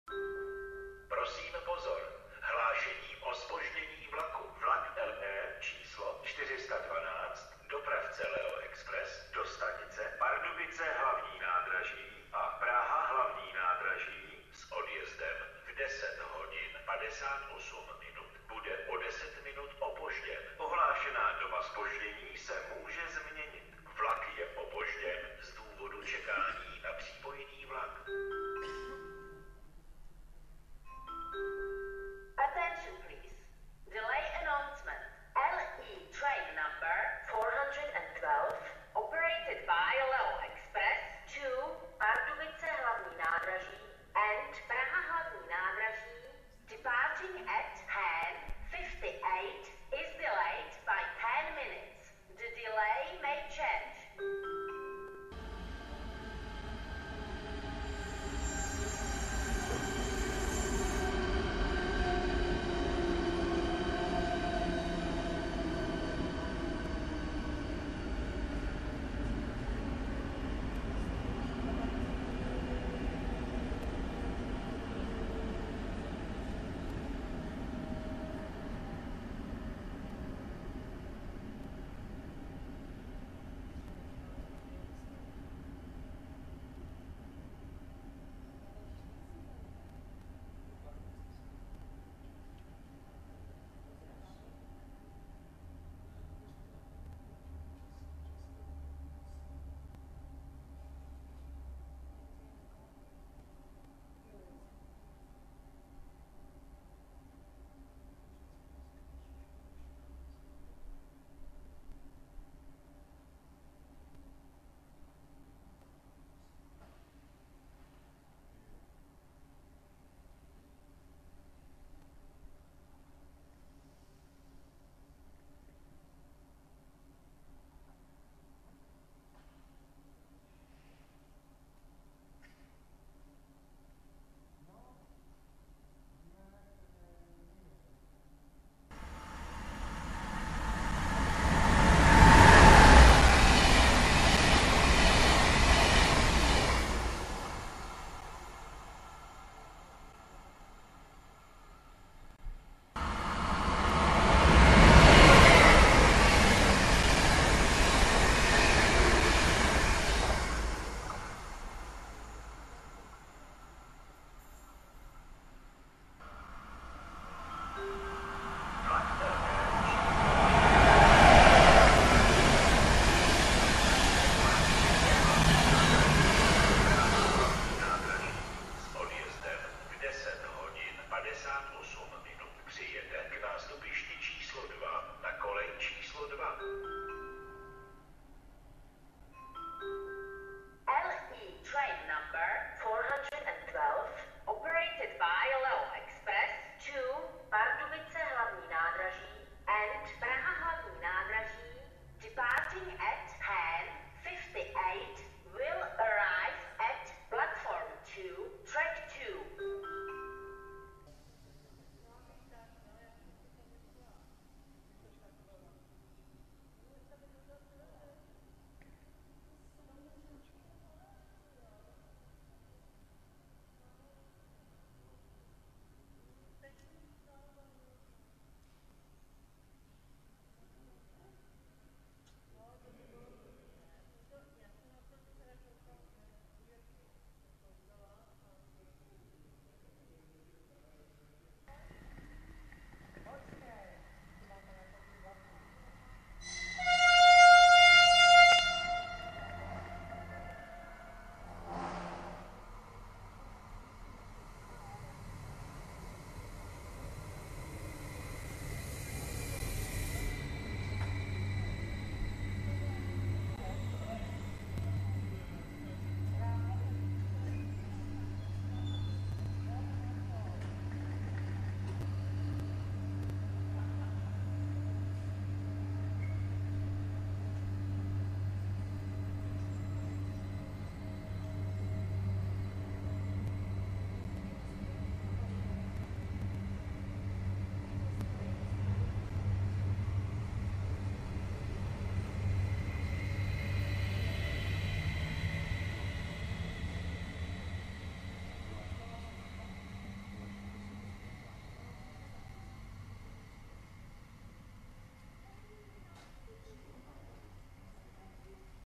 Hlášení a vlak Ústí nad Orlicí-LE 412+Zpoždění+Příjezd a Odjezd vlaku CityElefant,27.8.2023[HaVIS 3]
0:00 LE 412 Leo Express ( bude o 10 minut opožděn + EN )
1:08 Příjezd dvou jednotek CityElefant
2:20 Průjezd vlaku
2:33 Průjezd vlaku Regiojet
2:47 Průjezd vlaku + LE 412 Leo Express ( přijede + EN )
3:33 Odjezd dvou jednotek CityElefant
4:02 Příjezd vlaku LE 412 Leo Express + pozdrav
4:37 Odjezd vlaku LE 412 Leo Express